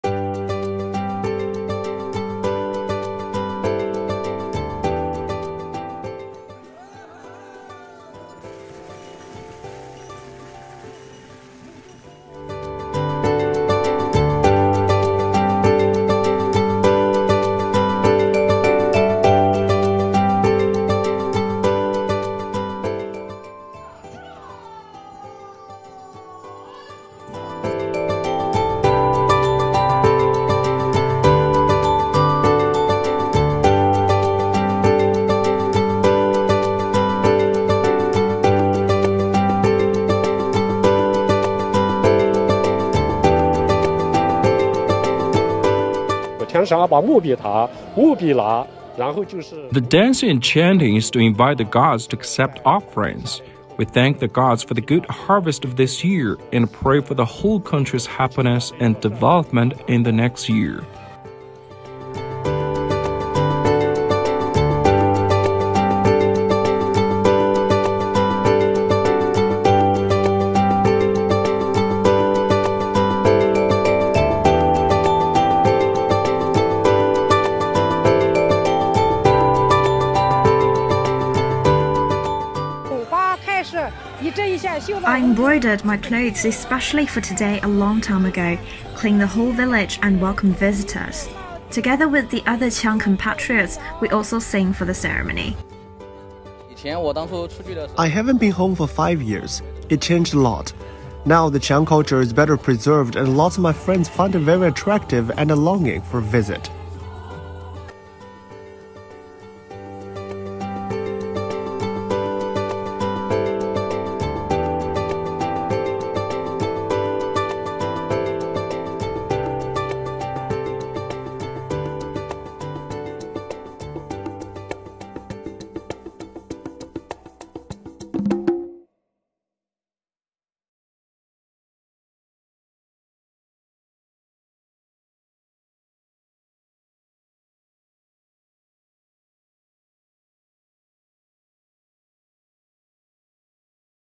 China's Qiang Minority Group Celebrates New Year
China's Qiang minority group in a village of southwest China's Sichuan Province has celebrated the Qiang New Year in the 10th lunar month with various colorful rituals and performances.